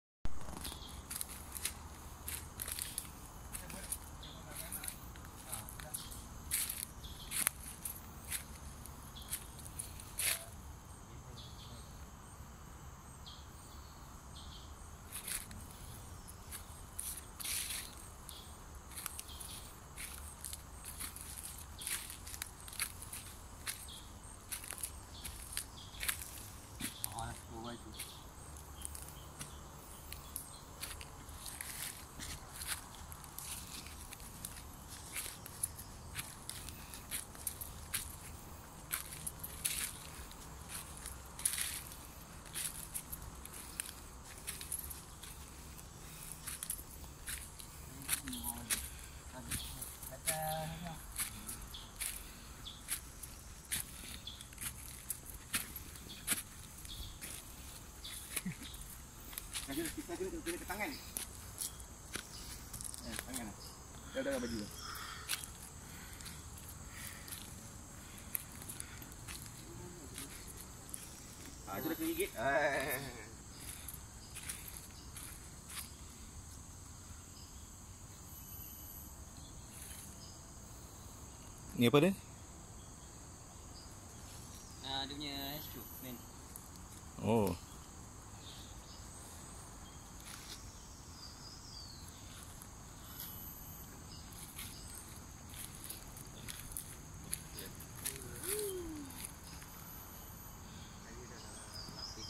WW2 trench Green Ridge at Kampar Perak
WW2 British communication trench line that connected B Company HQ bunker with Vickers machine gun position at Green Ridge battlefield in Perak Malaysia during WW2